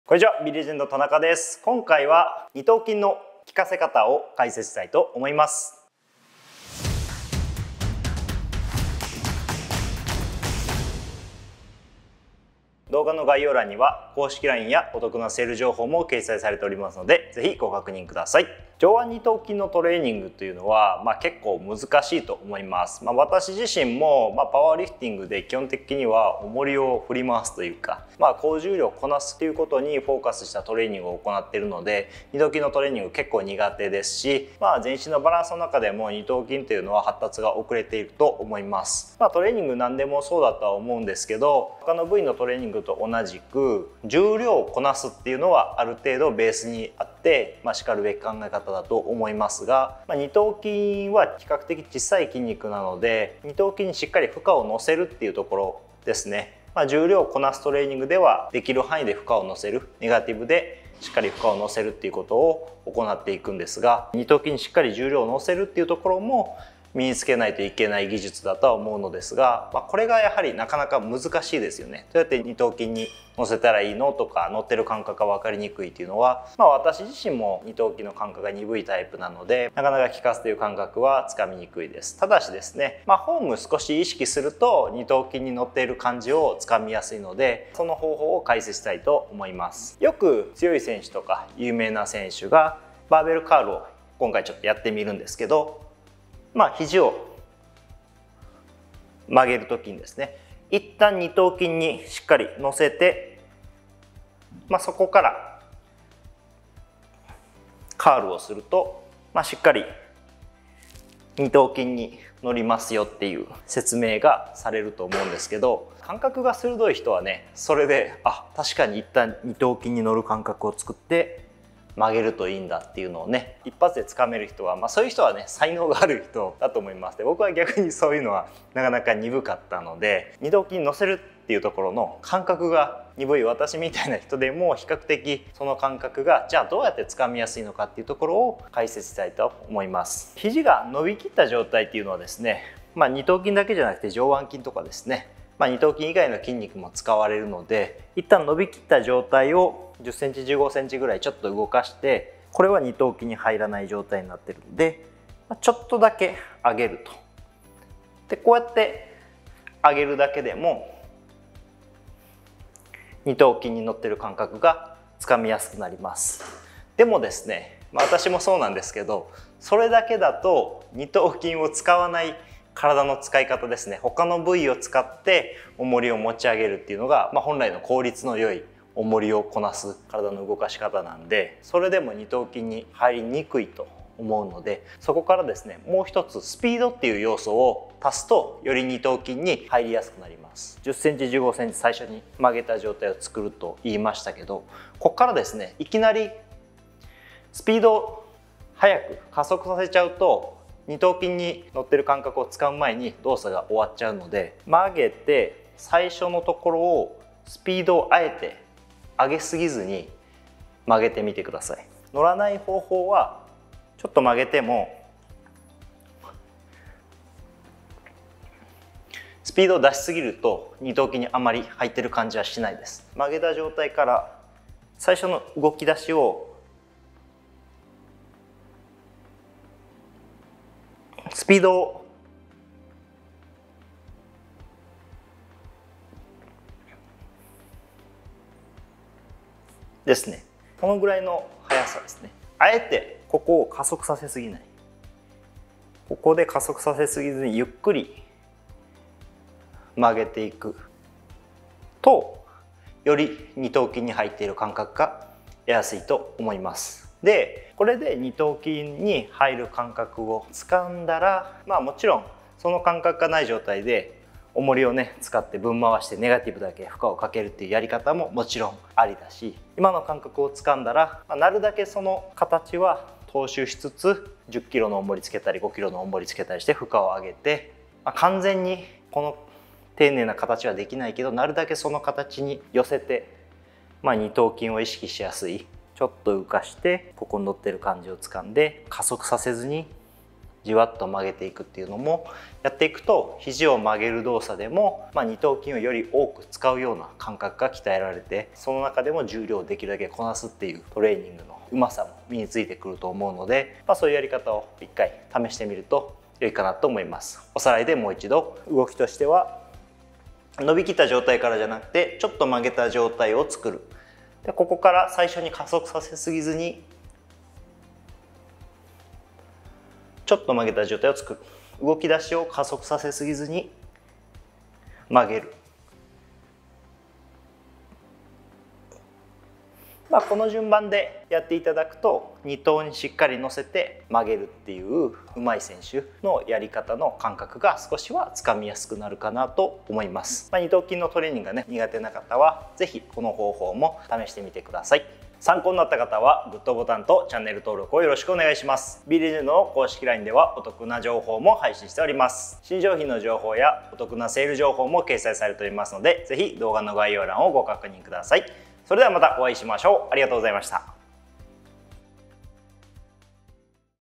【筋トレ】デカい力こぶを手に入れるためのポイント | パワーリフターが解説【ビーレジェンド プロテイン】
上腕二頭筋に「効いている感じがしない…」「負荷が乗っている感覚が分かりにくい…」という方もおられるのではないでしょうか？
フォームを少し意識するだけで負荷が乗っている感覚をつかみやくなります
今回は「デカい力こぶを手に入れるためのポイント」について解説します
■【数量限定】あっ！と驚くあずき風味
00:00 オープニング
00:13 デカい力こぶを手に入れるためのポイント